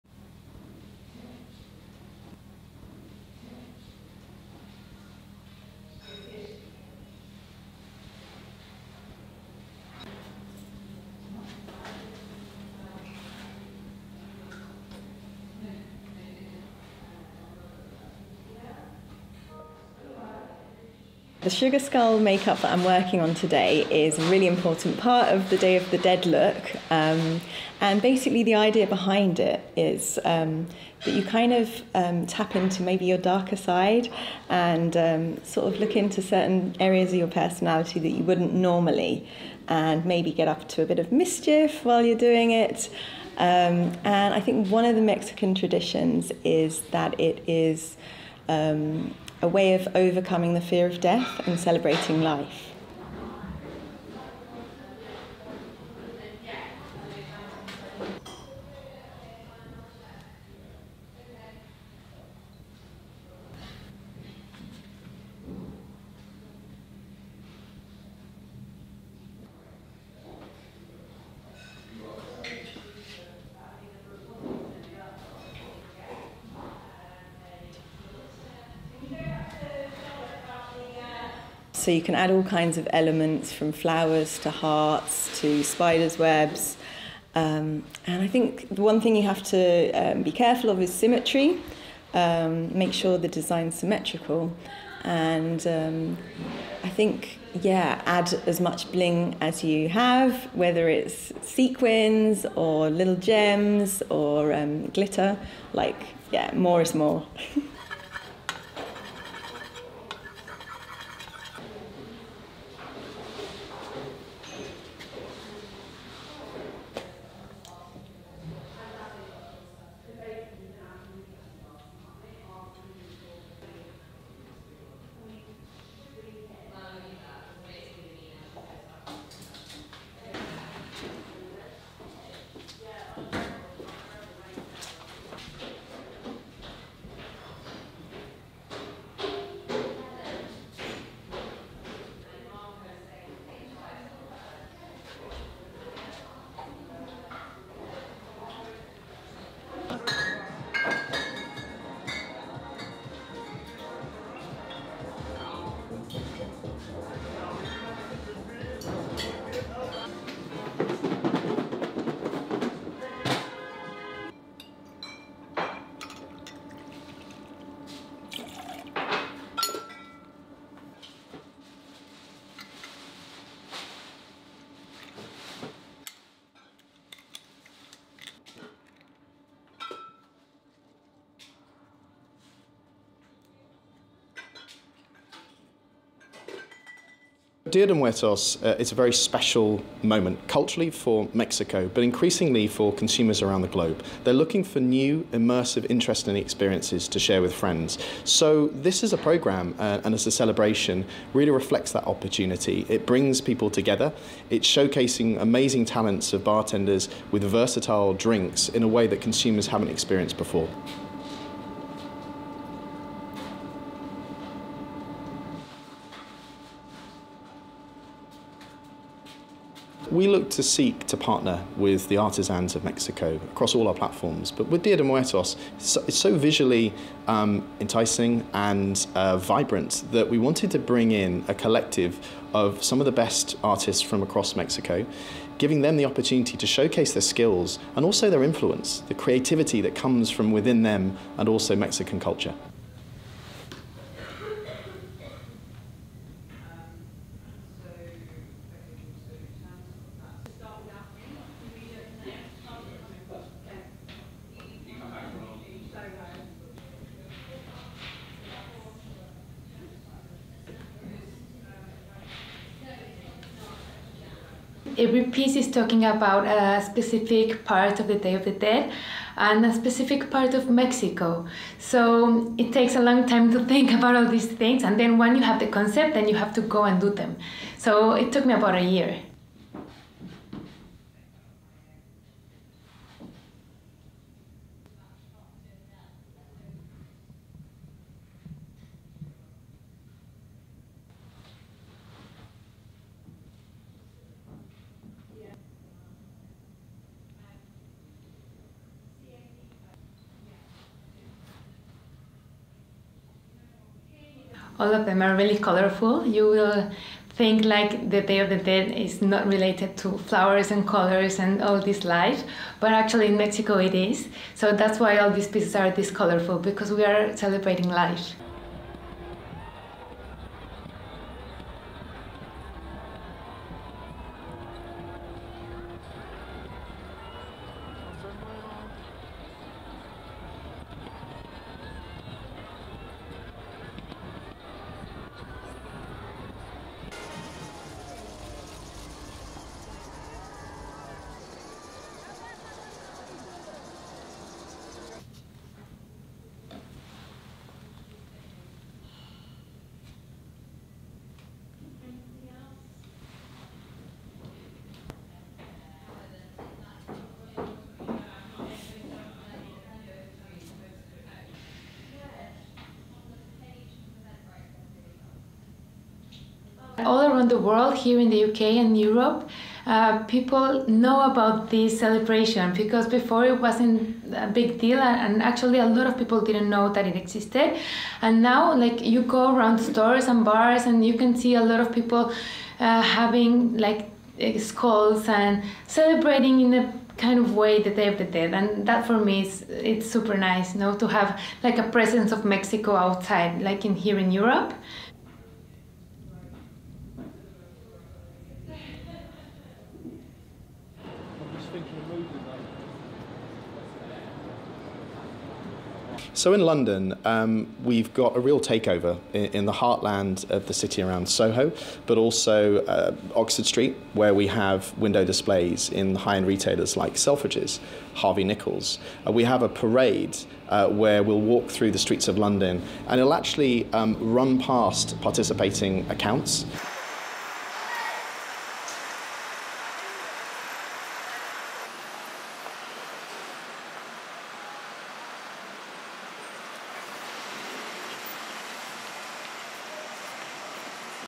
Mexican tradition global hit in the Instagram era
(30 Oct 2019) LEAD IN:
The Mexican celebration of the "Day of the Dead" is gaining enormous global following.
Now cities around the world, including London, are marking the festivities with everything from special drinks to colourful costumes and intricate face painting .

STORY-LINE:
The Day of the Dead - a time to shine and glorify in skulls, skeletons and alter-egos.
Its global popularity is attributed to Hollywood, and the 2015 James Bond film, "Spectre," in which Daniel Craig's title character dons a skull mask as he makes his way through a crowd of revellers.
But beyond the silver screen, the eye-catching festival in Mexico has also been recognised for its cultural significance with an inclusion on UNESCO's list of Intangible Cultural Heritage of Humanity.
The Day of the Dead is celebrated between the 31st of October and the 2nd of November each year, and is a time when Mexicans remember their deceased loved ones.
But the festival is also a global phenomenon, with scores of YouTube videos offering Day of the Dead makeup tutorials, as well as posts on Instagram and Pinterest.
Here in London, professional makeup artist Hazel Joanna has perfected the famously Instagrammable "sugar skull" look.
But she says the look is about more than simple cosmetics.
"The sugar skull makeup that I'm working on today is a really important part of the "Day of the Dead look", and basically the idea behind it is that you kind of tap into maybe your darker side and sort of look into certain areas of your personality that you wouldn't normally, and maybe get up to a bit of mischief while you're doing it. And I think one of the Mexican traditions is that it is a way of overcoming the fear of death and celebrating life," she explains.
Joanna says the key to perfecting the makeup is balance.
"So you can add all kinds of elements - from flowers, to hearts, to spiders' webs - and I think one thing you have to be careful of is symmetry - make sure the design's symmetrical - and I think add as much bling as you have, whether it's sequins or little gems or glitter, like, yeah - more is more," she adds.
A decade ago these colourful skulls hanging above a bar in London's Soho might have been mistaken for Halloween decorations.
But now, they are widely recognised for what they are: part of the Day of the Dead commemorations.
This year, Mexican brand Tequila Patron spotted its own global commercial opportunity by organising coordinated Day of the Dead events and parades.
"Dia de Muertos - it's a very special moment culturally for Mexico, but increasingly for consumers around the globe. They're looking for new, immersive, interesting experiences to share with friends. So this, as a programme and a celebration, really reflects that opportunity. It brings people together, it's showcasing amazing talents of bartenders with versatile drinks in a way that consumers haven't experienced before," says Matthew Sykes, Patron's Senior Director of Marketing.
Over the next few days, Patron's events will take place from Mexico City to Melbourne, the Philippines to LA, Belgium, Berlin, Paris and Cape Town.
In the UK alone, 300 bars will be involved in the coordinated celebrations, offering special Day of the Dead cocktails and Mexican food.
Sykes adds they wanted to celebrate Mexico and the Day of the Dead in an authentic way, and so brought traditional artisans to Europe from Mexico to get involved.
Lourdes Villagomez is one of Mexico's most famous contemporary artists, and one of the five artists from Mexico City chosen for the events.
Villagomez says her pieces have been carefully planned.